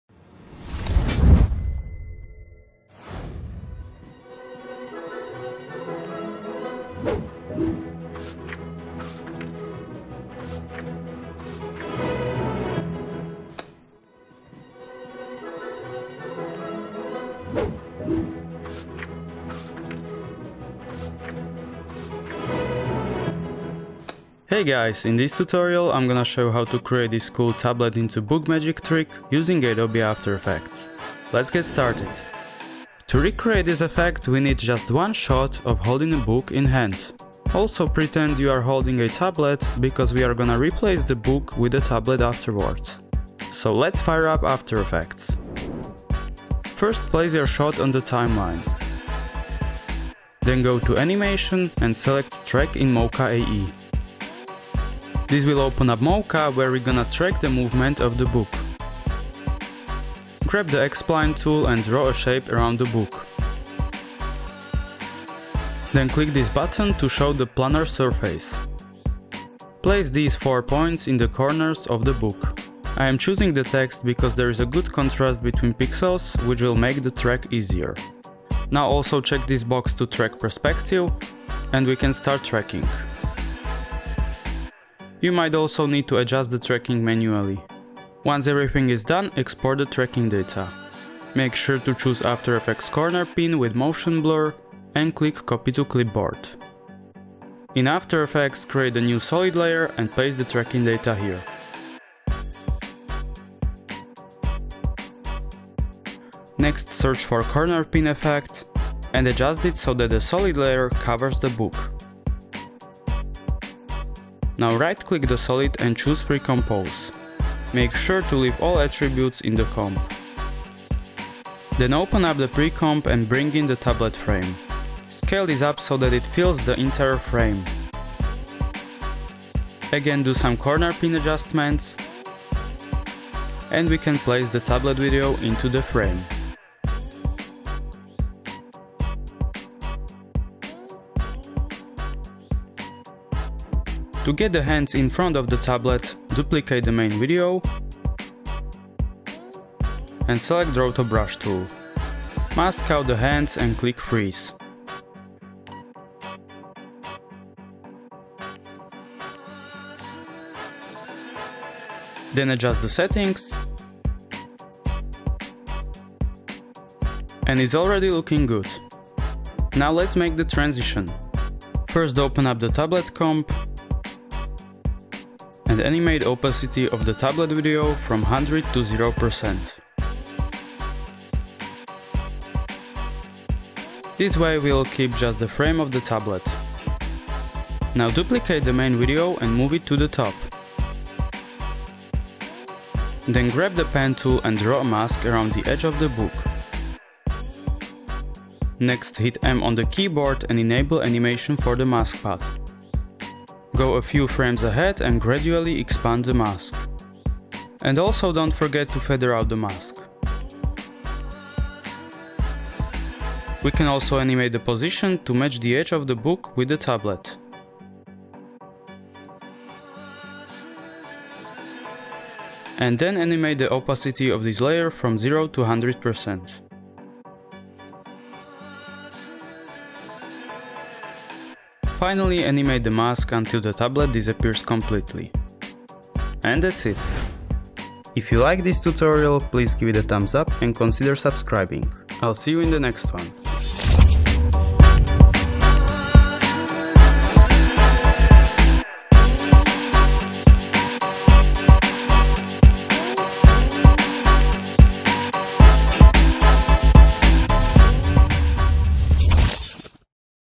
TURNING TABLET INTO BOOK (Zach King Style) - After Effects VFX Tutorial
⚡Unlimited downloads of millions of templates, music, stock footage and video editing assets.
▶In this tutorial, I will show you how to make Turning Tablet into Book - Magic Trick in Adobe After Effects. Learn how to edit like Zach King.